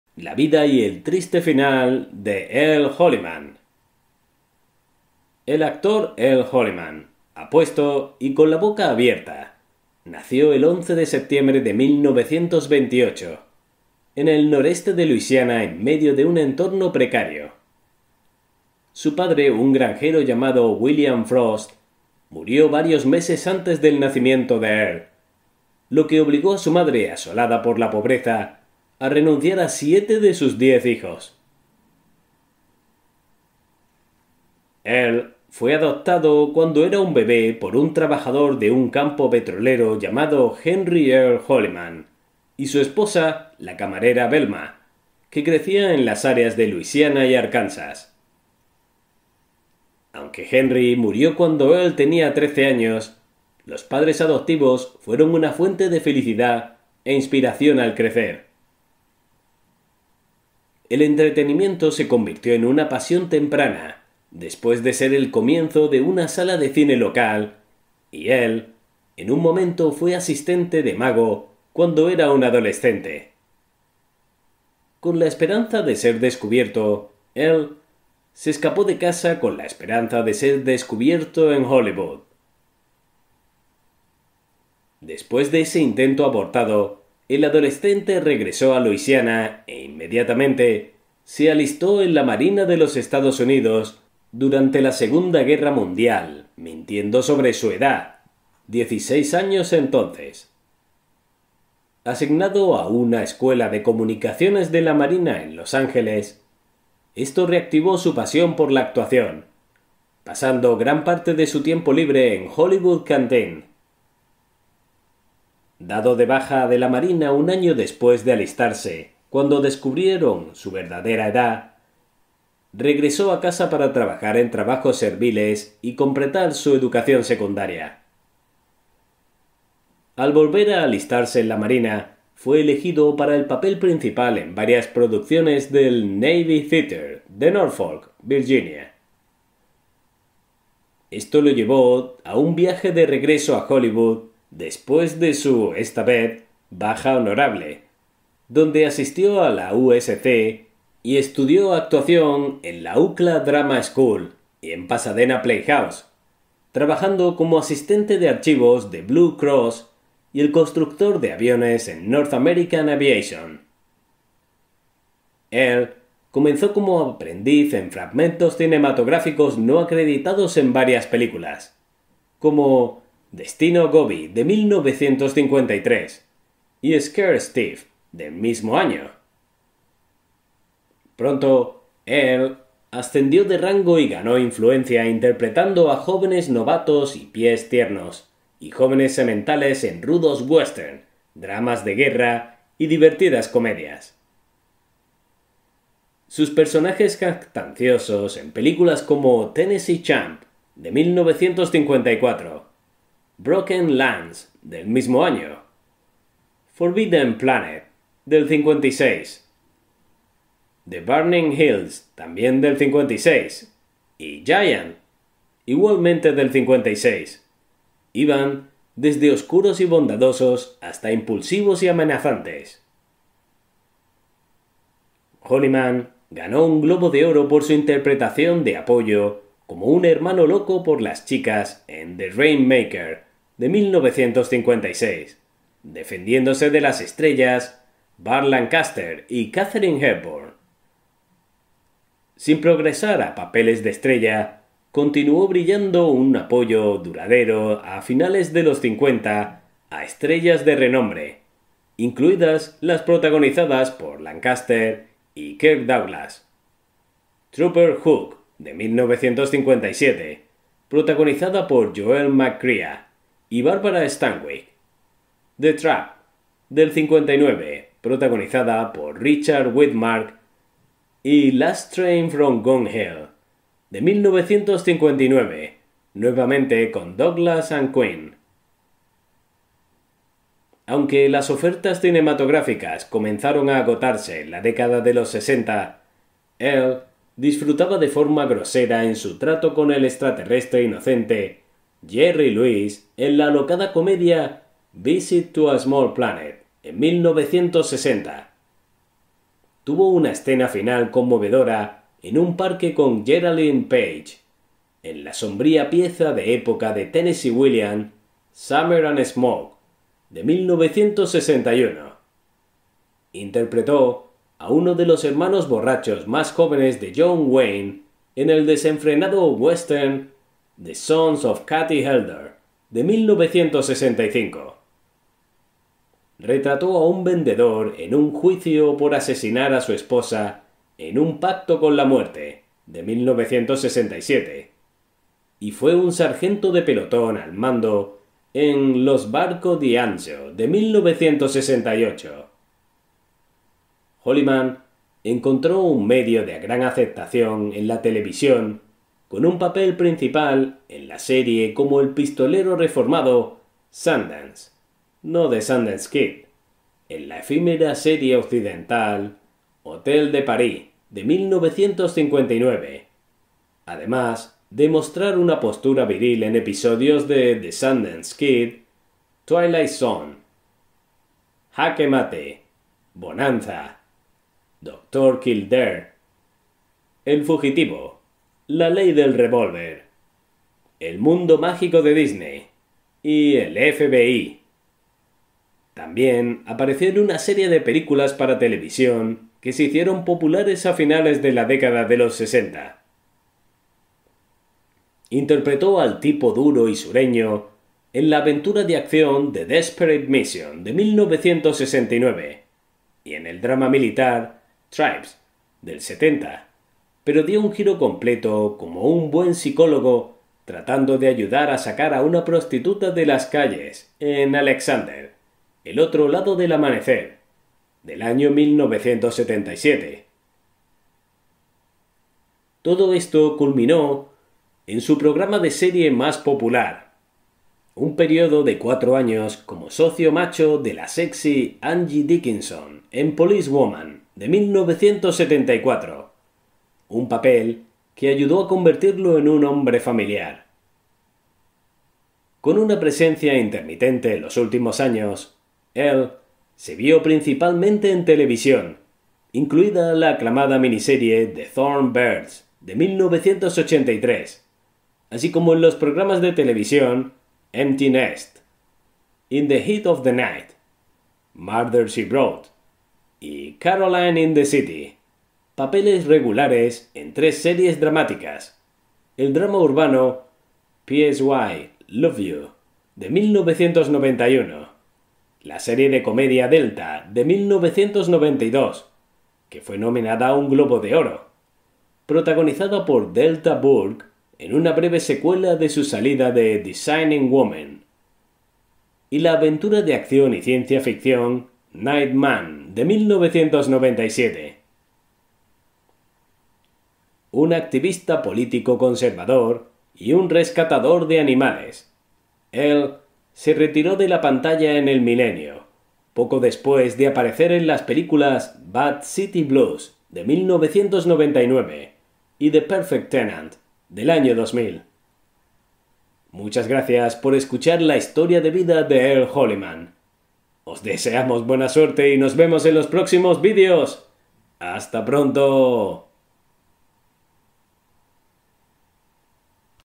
La Vida y El Triste Final de Earl Holliman
Mini biografía de la vida de Earl Holliman. Henry Earl Holliman (nacido el 11 de septiembre de 1928) es un actor, activista por los derechos de los animales y cantante estadounidense conocido por sus muchos papeles de personajes en películas, principalmente westerns y dramas, en las décadas de 1950 y 1960. Ganó un Globo de Oro por la película The Rainmaker (1956) e interpretó al sargento Bill Crowley en el drama policial de televisión Police Woman a lo largo de su carrera de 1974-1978.